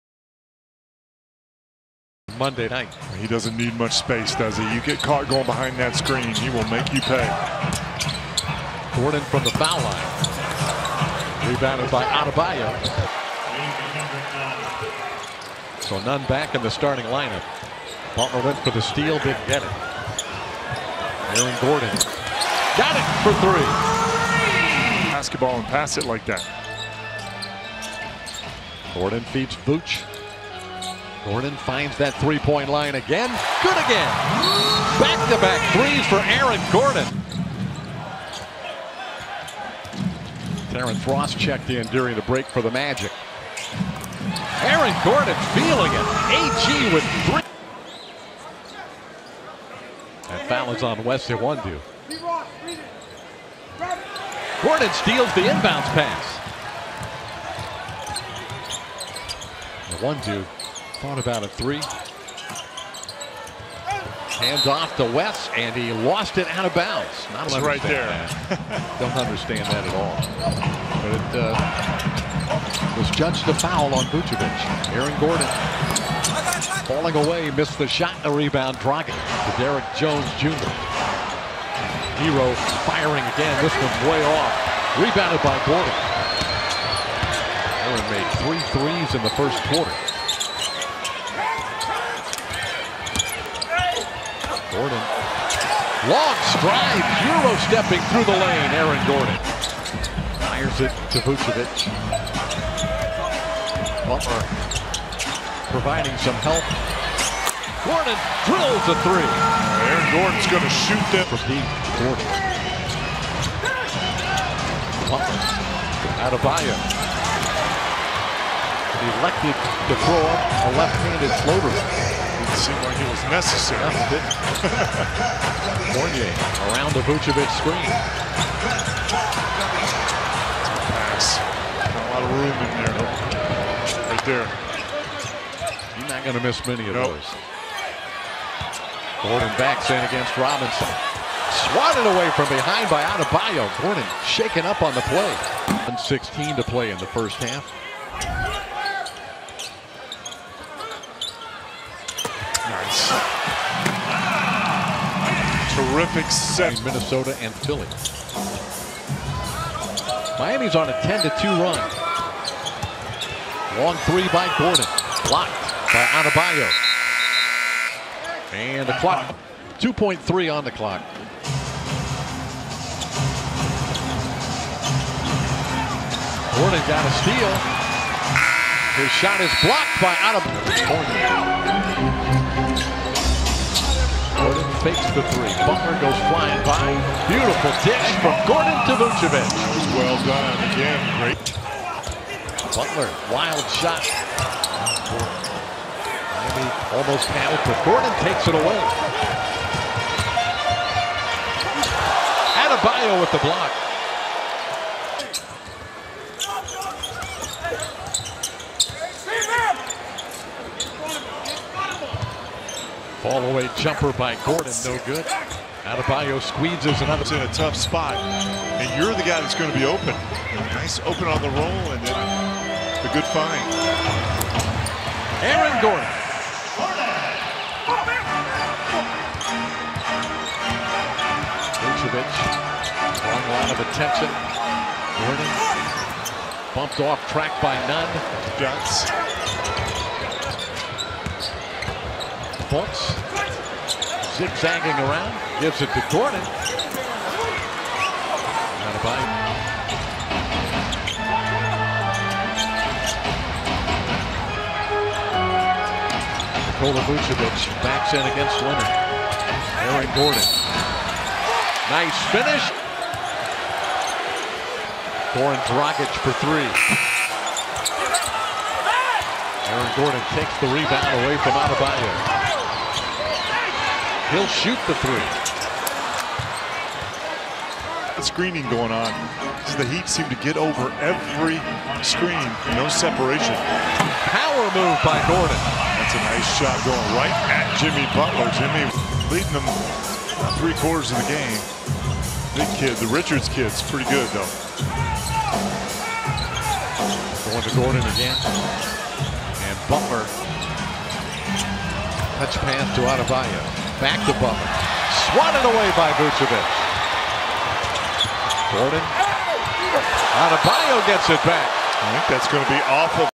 Aaron Gordon Full Play vs Miami Heat | 02/01/20 | Smart Highlights
Miami Heat 102 - 89 Orlando Magic